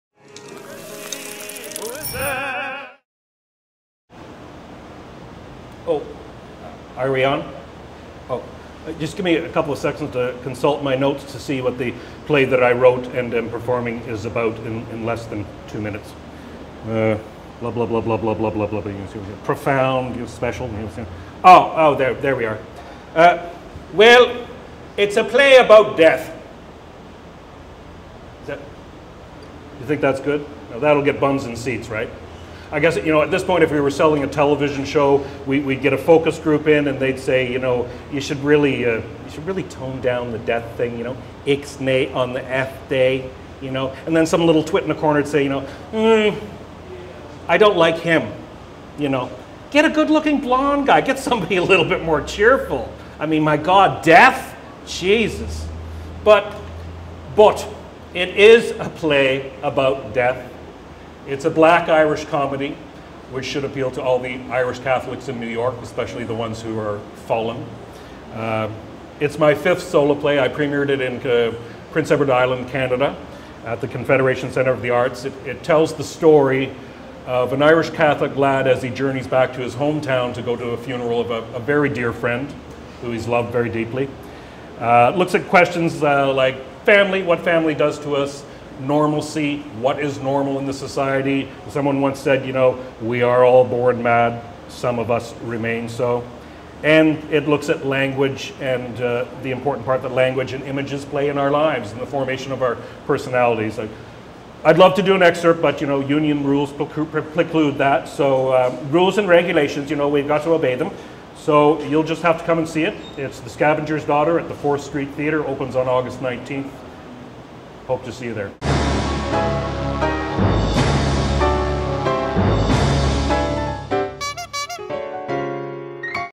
The Scavenger's Daughter
Colm Magner describes his show "The Scavenger's Daughter" in under two minutes!